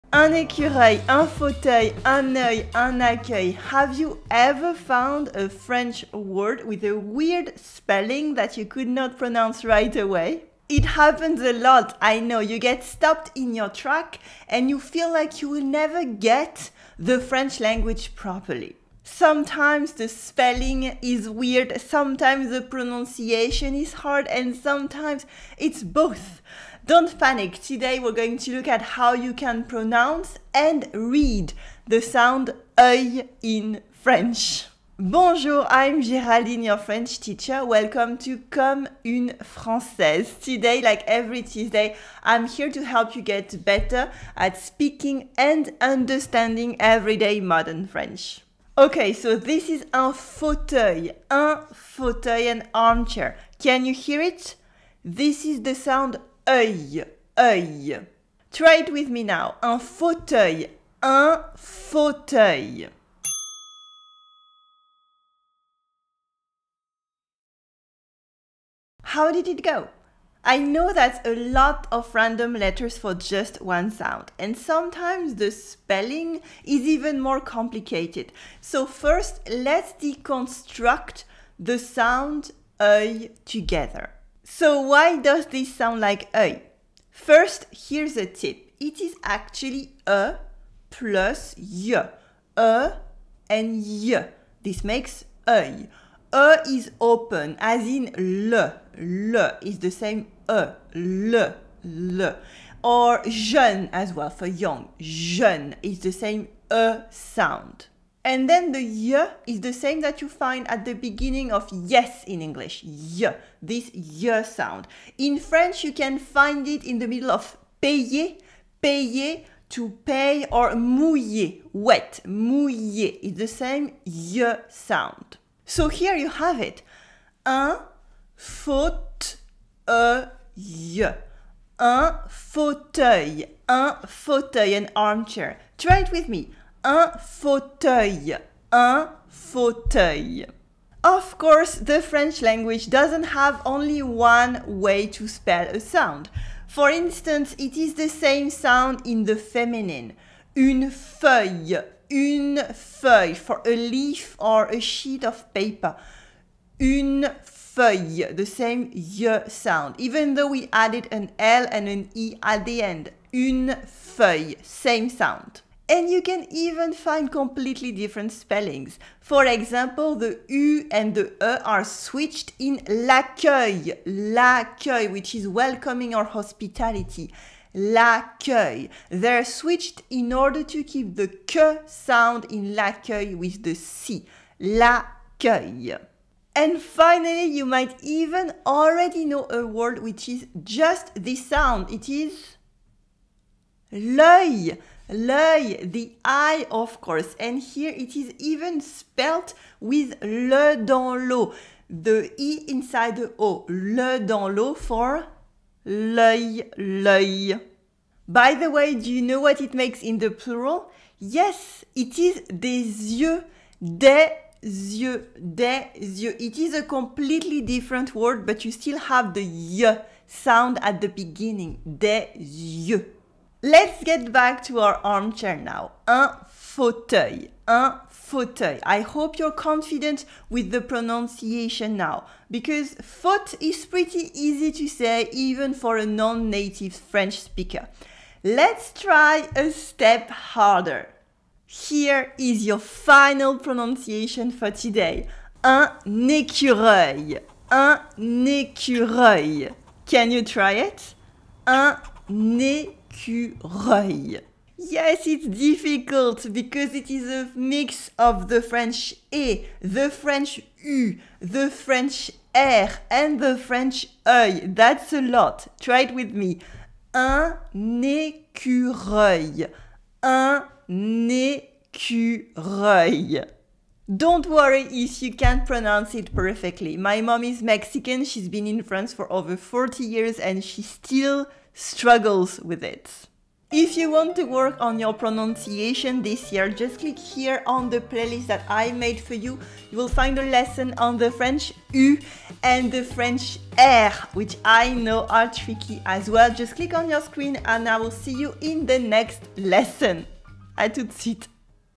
How to pronounce the “euil” sound in French
How do you pronounce fauteuil in French? How about écureuil, or accueil? Each of these words use the weird French sound “euil”...

It’s no secret that many French words have weird pronunciation. And one sound that always makes French learners pause is the “euil” sound. You’ll see it in words like un fauteuil (an armchair), un écureuil (a squirrel), and un accueil (welcome / hospitality / an entrance lobby).

In today’s lesson, I’ll show you some tricks for getting around this weird French pronunciation, so you can get better at speaking real, everyday French! Let’s go!

😘 from Grenoble, France.

Géraldine